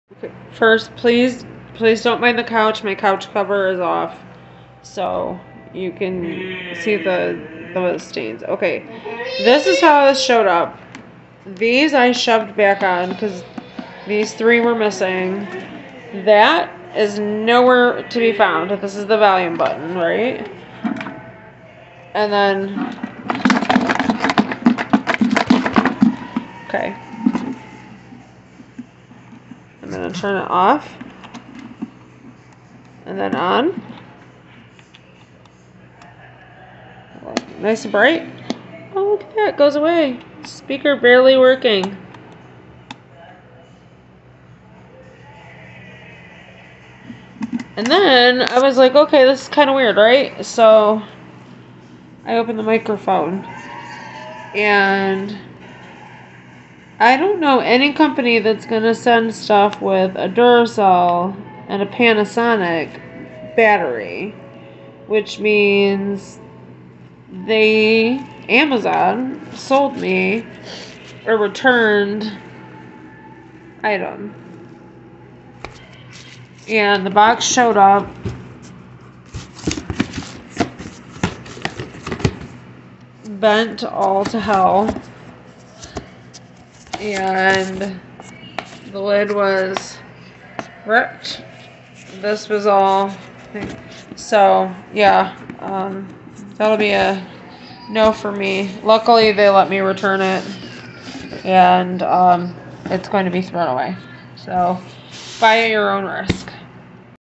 MASINGO 2023 Bluetooth Karaoke Machine for Kids and Adults with 1 Wireless Karaoke Microphone and 1
Karaoke Machine for Kids and Adults with 1 Wireless Karaoke Microphone and 1 Wired Mic, PA Portable Speaker System with LED Lights, Supports TF Card/USB, AUX/MIC in, TWS for Home Party, Burletta C10

Burletta C10 Karaoke Machine Review: Your Portable Home Singing Solution

Singing typically brings joy to people of all ages, and karaoke is one of the best ways to showcase your singing talents and have fun with friends and family. But finding the perfect karaoke machine is no easy task. That's why we decided to make this review of the Burletta C10 karaoke machine. Here we'll go through its specs and features, and provide an honest opinion on whether it's the right karaoke solution for you